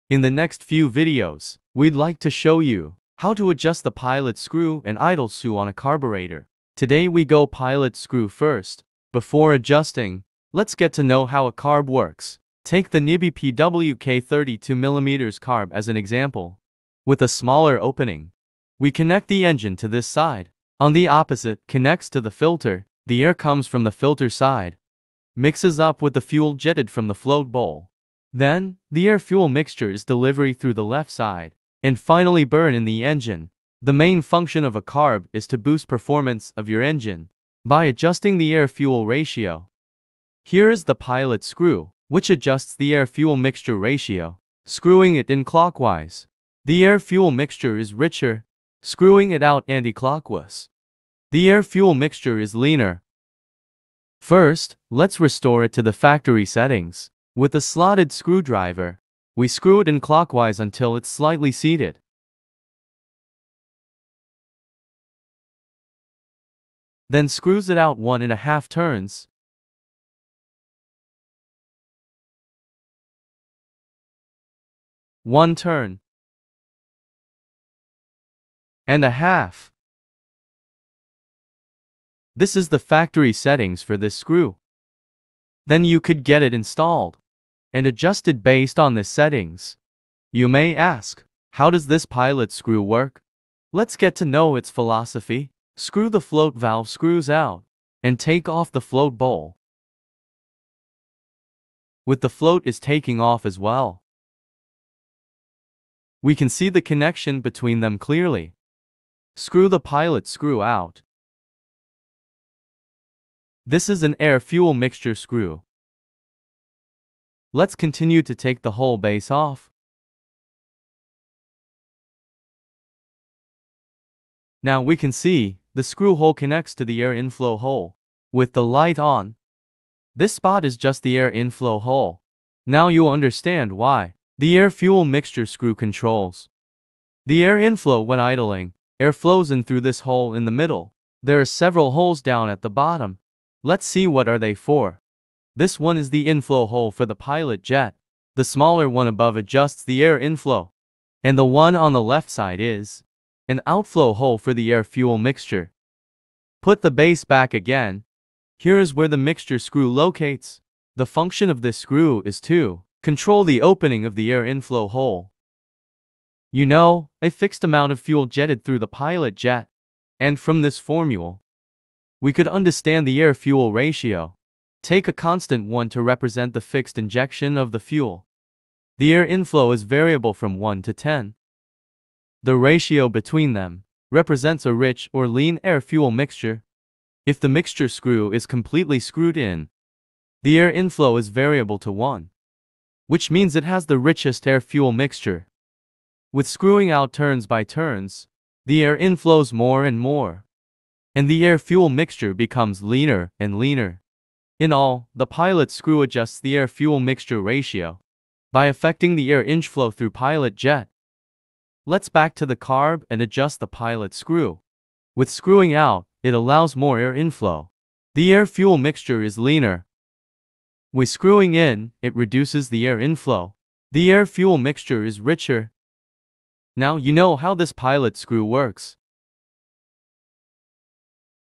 Pilot & Idle Screw Adjustment Explained | Part 1 - Pilot/Air-Fuel Mixture Screw
My bike seems running too rich/lean, how do I adjust the ratio to get the mixture well and run better? How does the pilot/air-fuel mixture screw work? If you have confused with these questions, this video will get you covered!